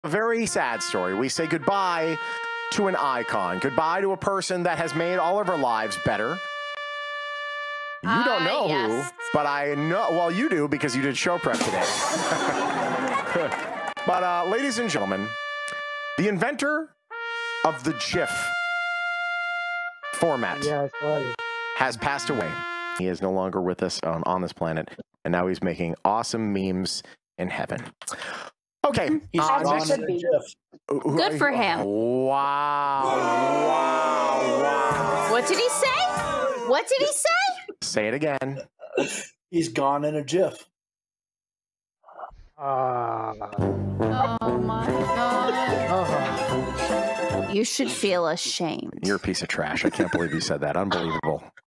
GIF Inventor Died | Free4All | 03/25/2022
RIP.gif

The inventor of the GIF file format has died.